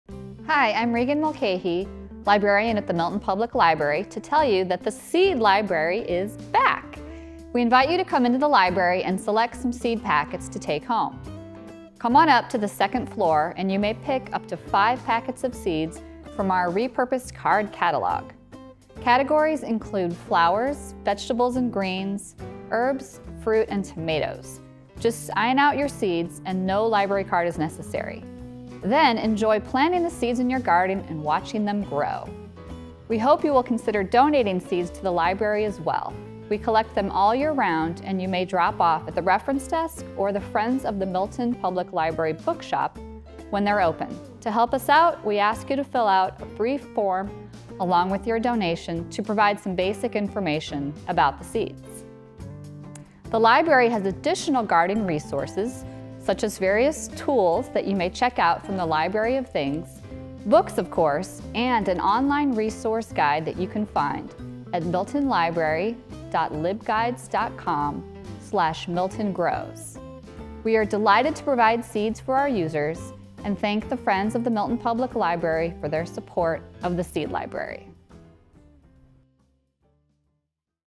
MPL Shorts: Free Seeds for You To Grow!
The Milton Public Library Seed Library is back! And MPL Technology Librarian Regan Mulcahy is back too, with all the info you need to get growing.

Milton, MA